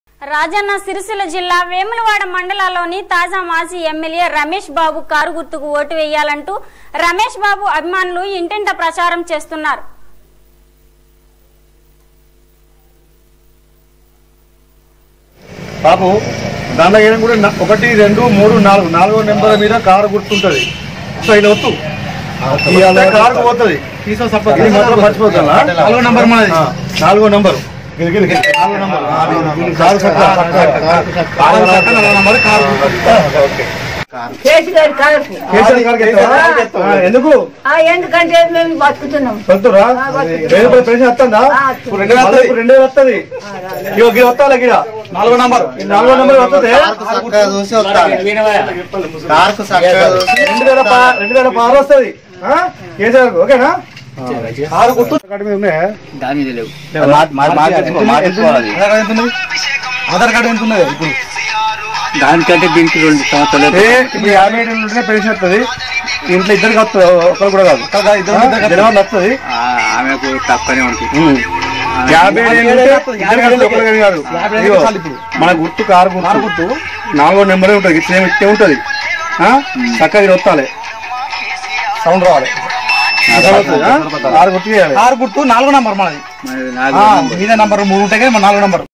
వేములవాడ మండలాల్లోని ఎమ్మెల్యే రమేష్ బాబు గారి కారు గుర్తుకు ఓటు వేయాలంటూ ప్రచారం
రాజన్న సిరిసిల్ల జిల్లా వేములవాడ మండలాల్లోని తాజా మాజీ  ఎమ్మెల్యే రమేష్ బాబు గారి కారు గుర్తుకు ఓటు వేయాలంటూ రమేష్ బాబు గారి అభిమానులు ఇంటింటా ప్రచారం చేస్తున్నారు